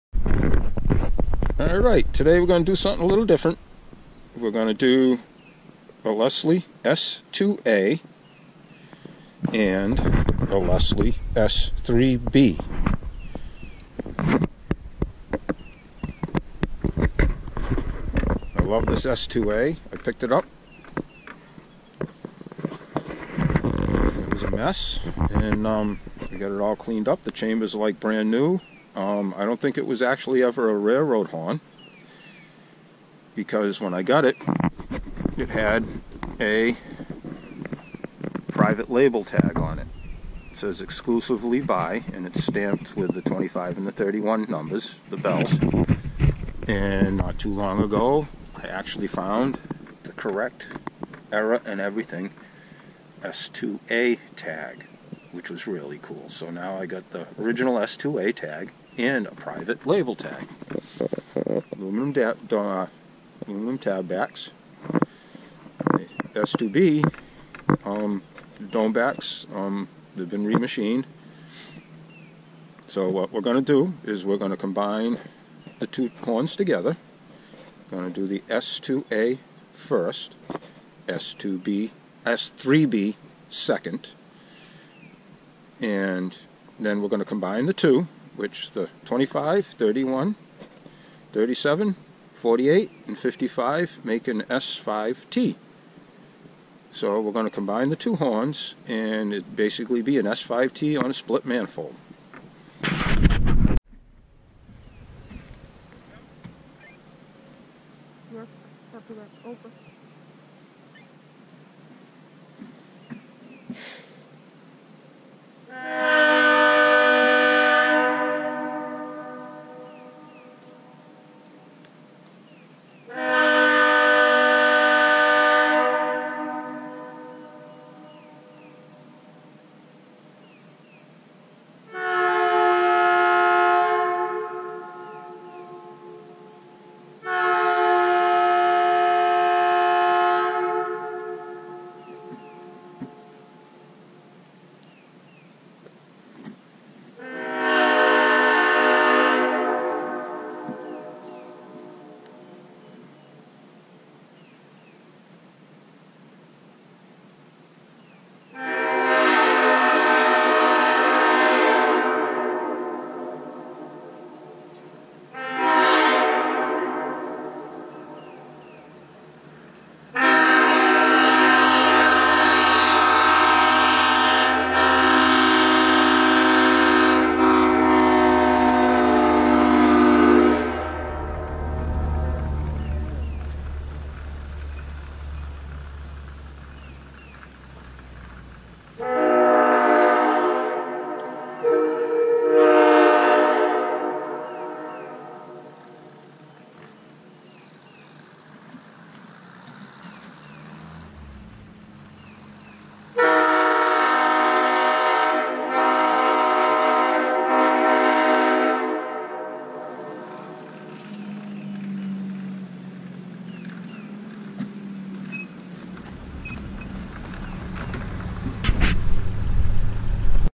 Leslie S-2A, S-3B then combined = Leslie S-5T
Leslie S-2A bells 25, 31 first. Leslie S-3B bells 37,44,55 second. Then combined to form a Leslie S-5T on a split manifold.